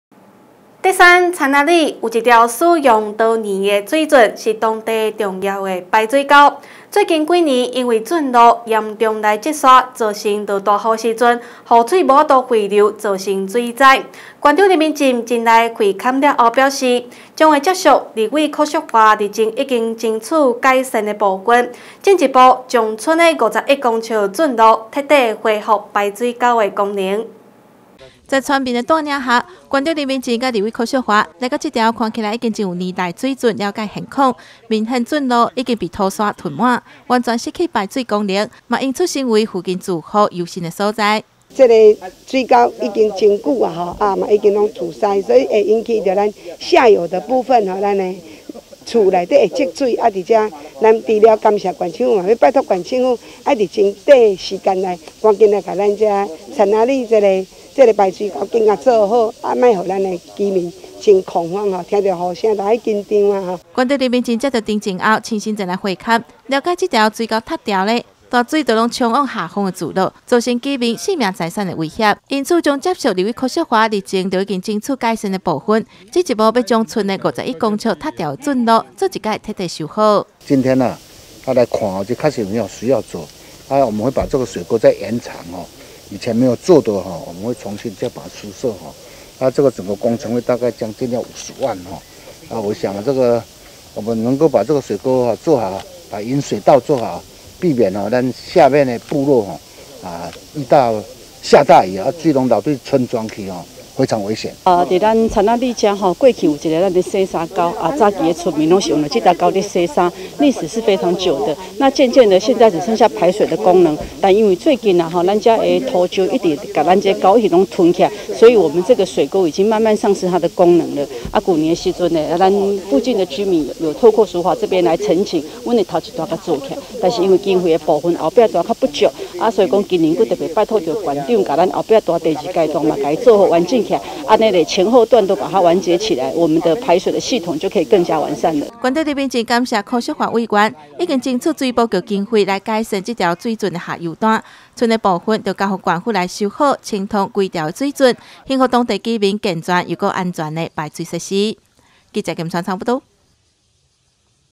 竹山有條古圳嚴重積砂 無法排水危及十餘戶 南投新聞
竹山田子里有一條使用多年的水圳，是當地重要的排水溝，最近幾年來已經因為圳路的嚴重淤積，造成下大雨時，雨水無法匯流造成水災。縣長林明溱前來會勘後表示，將接續立委許淑華日前已經爭取改善的部分，進一步將剩下的51公尺淤積圳路，做一次徹底恢復排水溝功能。